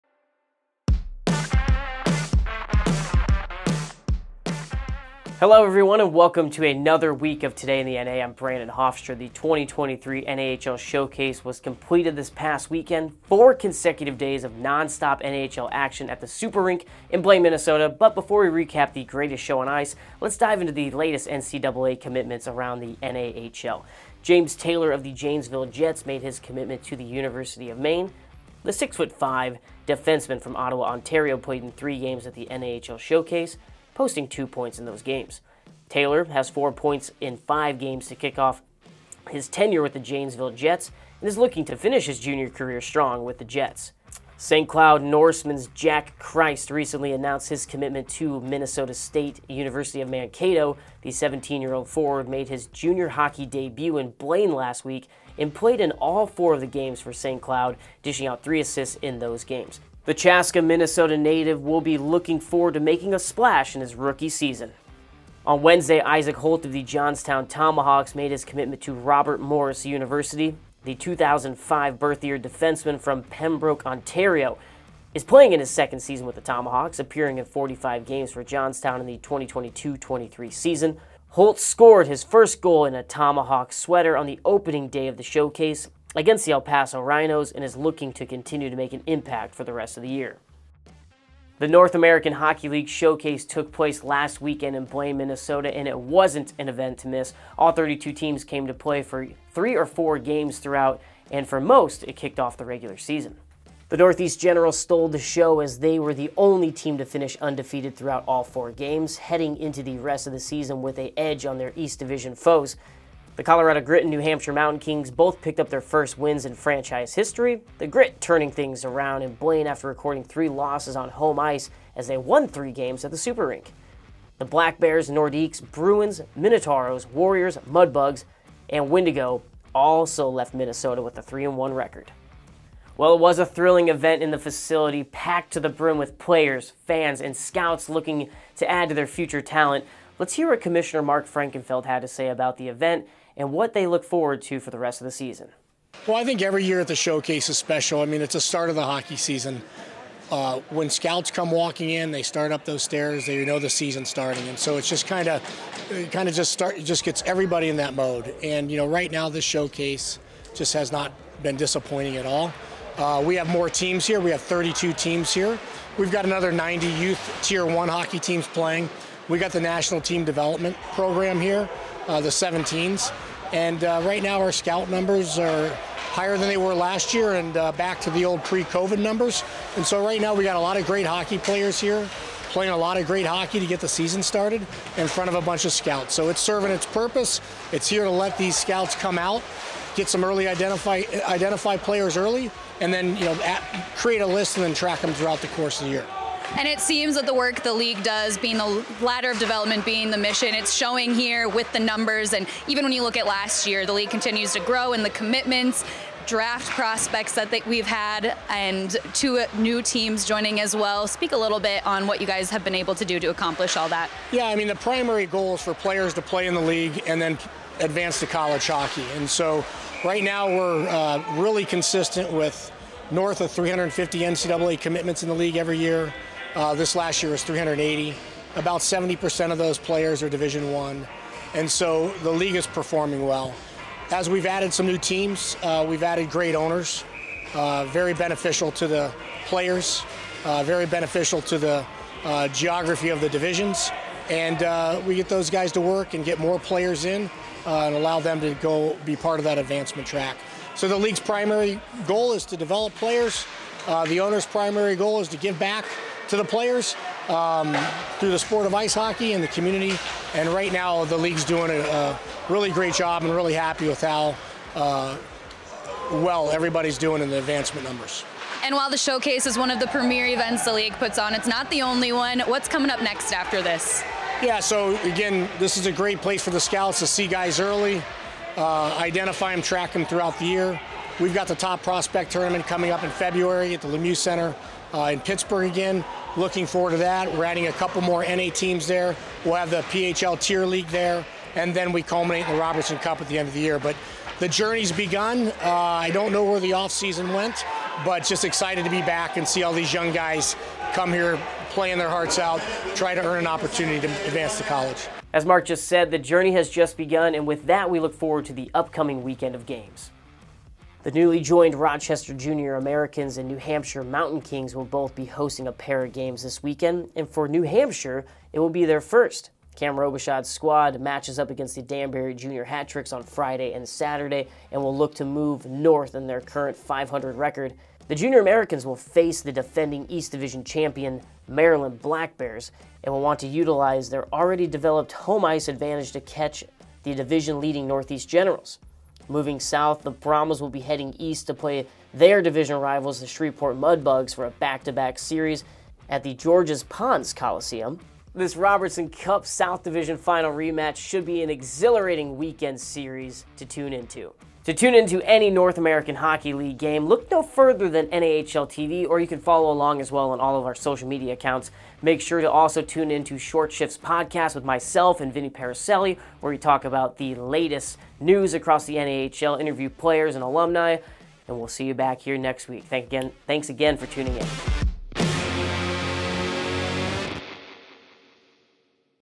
Today in the NA - 9/22/23
This week on Today in the NA we recap the NAHL Showcase, touch on the most recent NCAA Commitments, and preview the upcoming weekend!